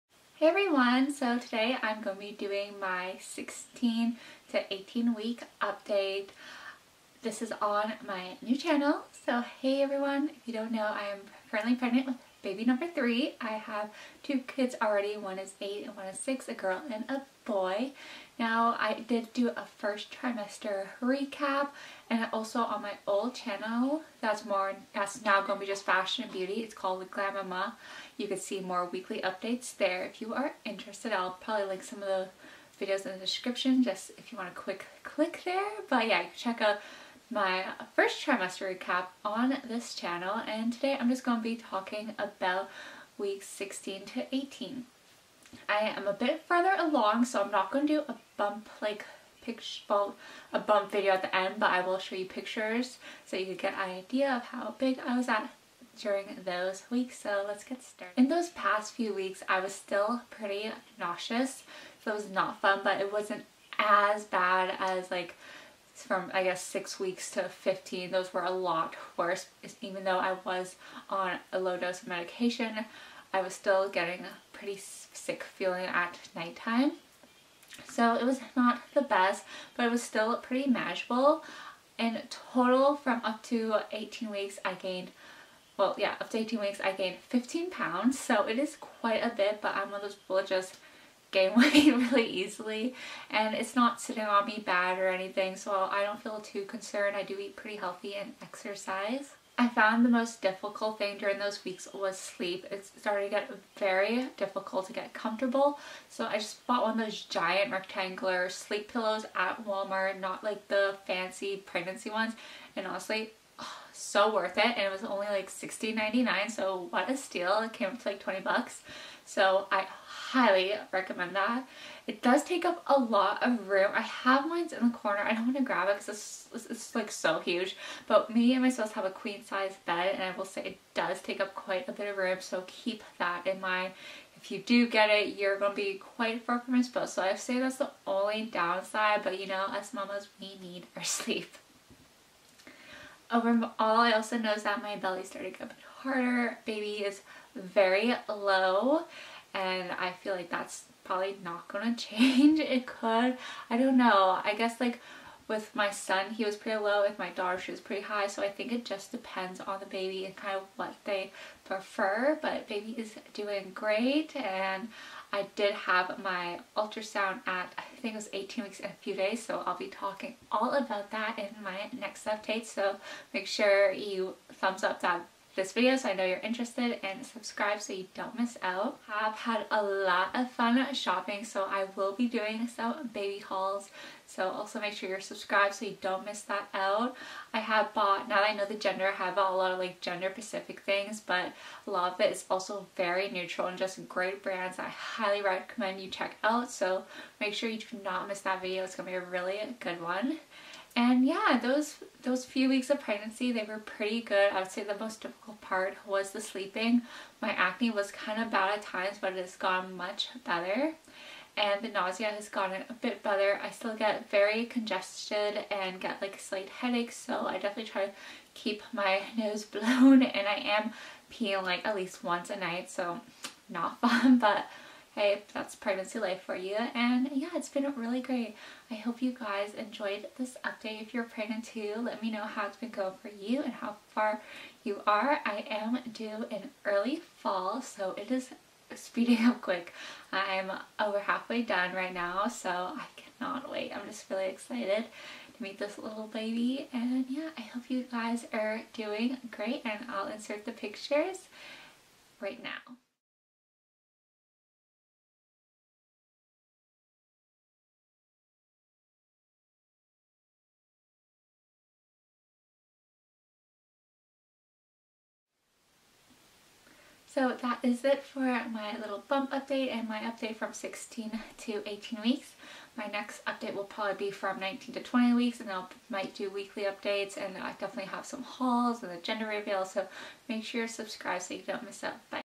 16-18 Week Pregnancy Update
Hey everyone I’m Jess I’m expecting my third baby in early Fall! :) I have a son and a daughter.

Beauty, fashion & luxury channel is glammama